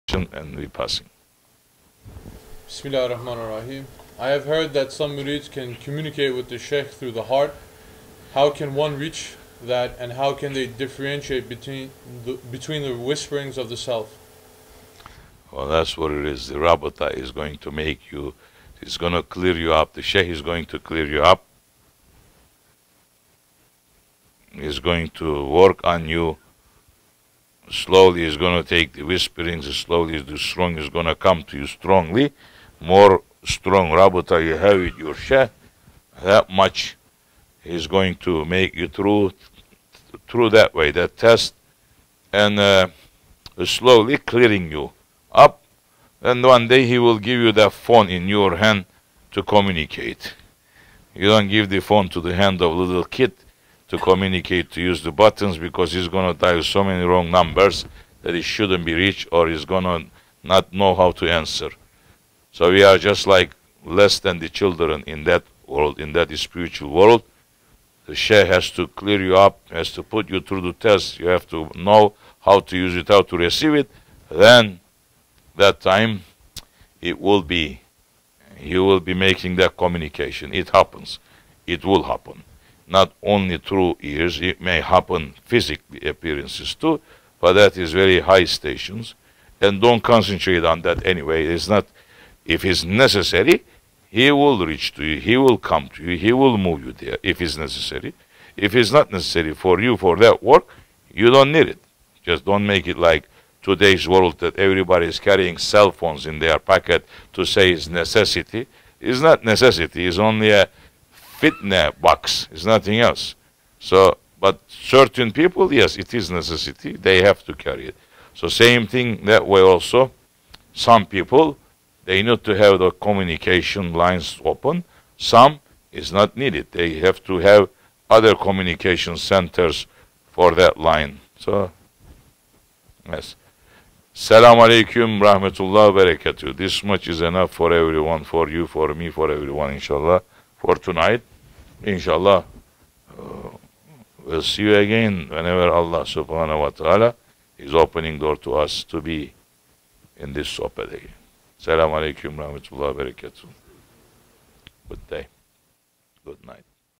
How to communicate with the Shaykh through the heart - Shaykh Abdulkerim el Kibrisi
Question: I have heard that some murids can communicate with the Shaykh through the heart. How can one reach that and how can they differentiate between the whispering of the self ?